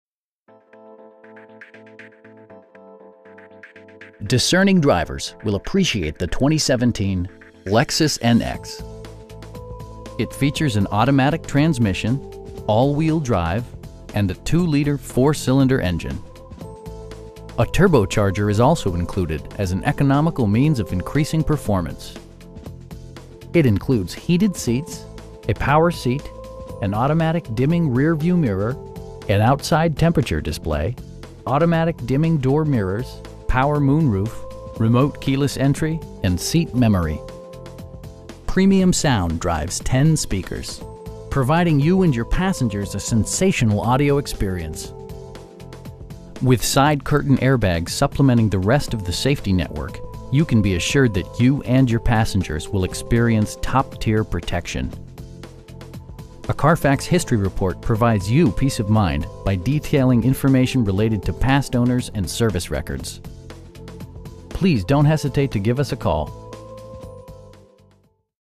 2017 Lexus NX NX Turbo F Sport in Concord, NC 28027
Discerning drivers will appreciate the 2017 Lexus NX. It features an automatic transmission, all-wheel drive, and a 2 liter 4 cylinder engine. A turbocharger is also included as an economical means of increasing performance. It includes heated seats, a power seat, an automatic dimming rear-view mirror, an outside temperature display, automatic dimming door mirrors, power moon roof, remote keyless entry, and seat memory. Premium sound drives 10 speakers, providing you and your passengers a sensational audio experience. With side curtain airbags supplementing the rest of the safety network, you can be assured that you and your passengers will experience top-tier protection. A Carfax history report provides you peace of mind by detailing information related to past owners and service records. We pride ourselves in consistently exceeding our customer's expectations. Please don't hesitate to give us a call.